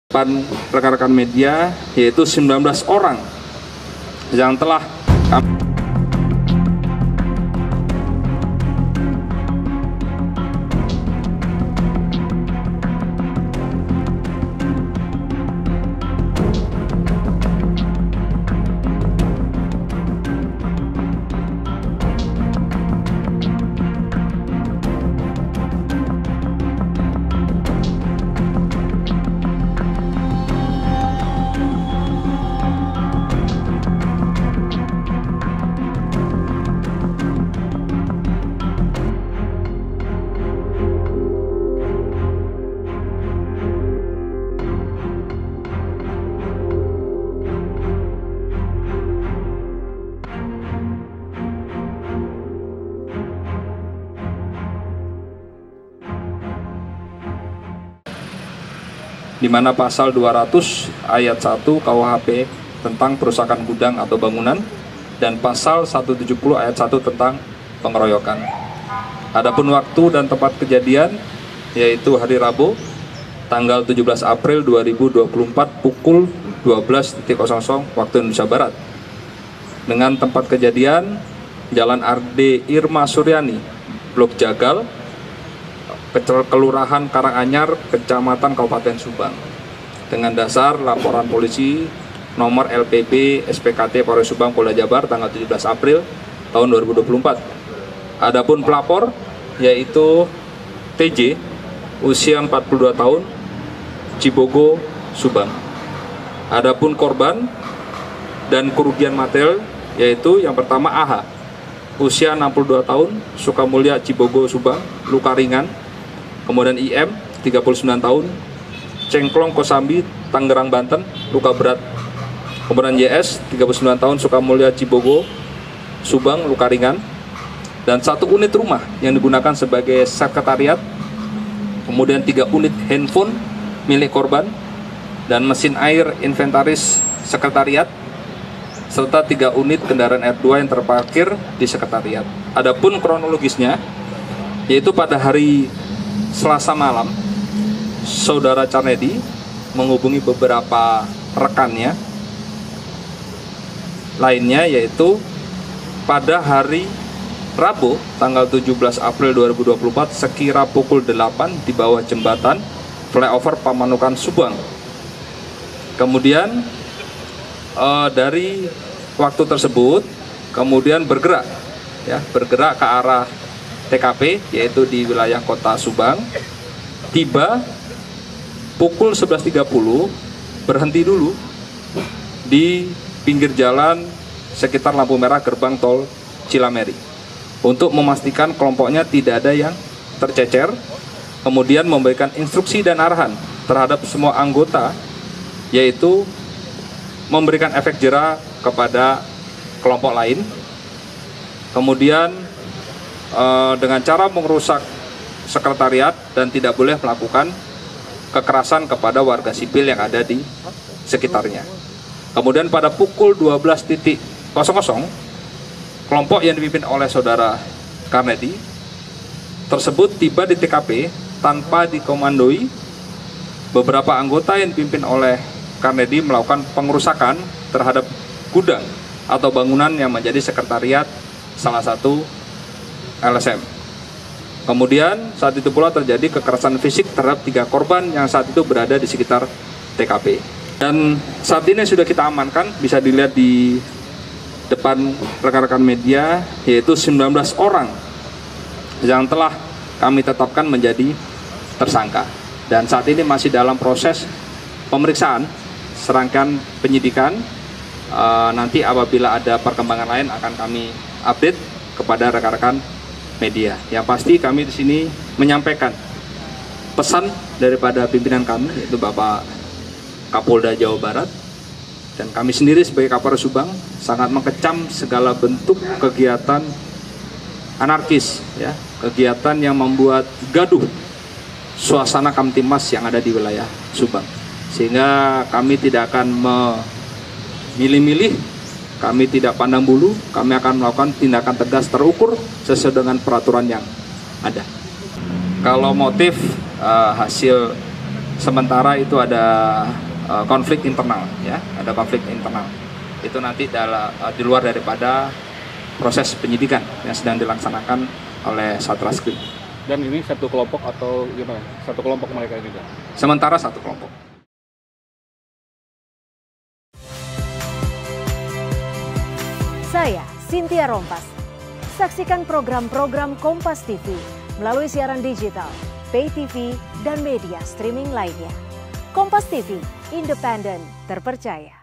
Rusuh Ormas Berkelahi, 19 Orang Jadi Tersangka
KOMPAS.TV - Rusuh Ormas Berkelahi di Subang, 19 Orang Jadi Tersangka